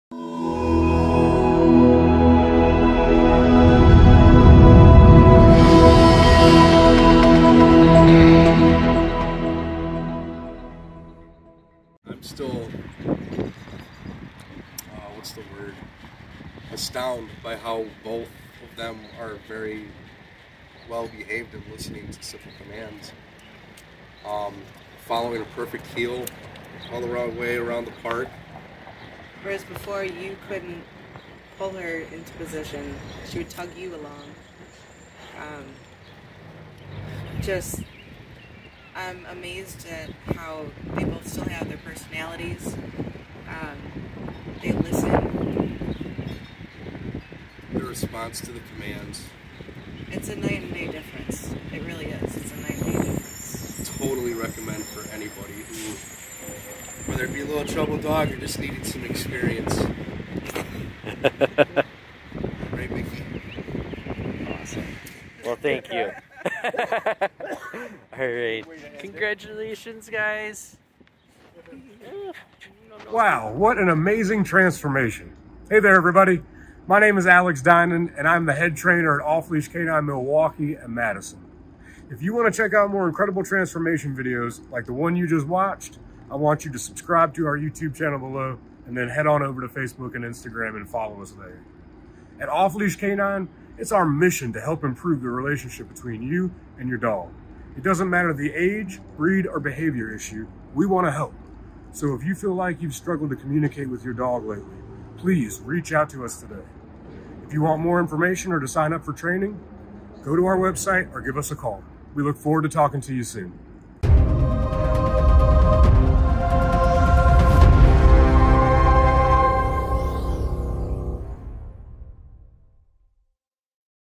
1 Y/O & 4 Y/O Siblings “Athena & Mickey” / Owner Testimonial / E-Collar Training
Looking for a heartwarming story of two furry siblings learning to be obedient and well-behaved? Look no further than Athena and Mickey, a one-year-old and four-year-old pair of pups who transformed into amazing listeners with the help of e-collar training.

In this video, their owner shares their journey to becoming the best-behaved dogs on the block, and the remarkable progress they've made. Join us for a touching tale of dedication, training, and love.